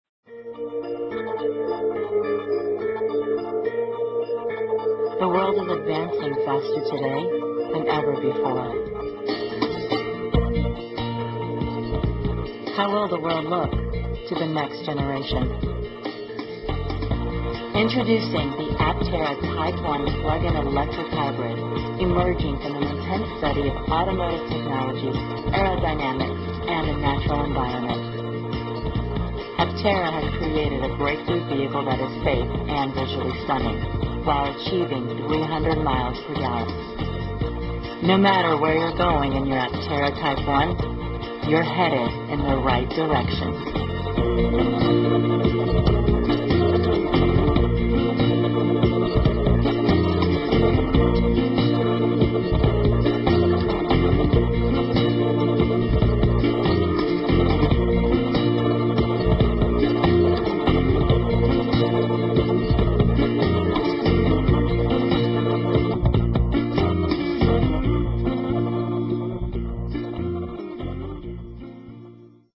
The Aptera typ-1
"According to Fambro, the Typ-1 is capable of 80 mph and could travel up to 70 miles on a single charge while sustaining that speed"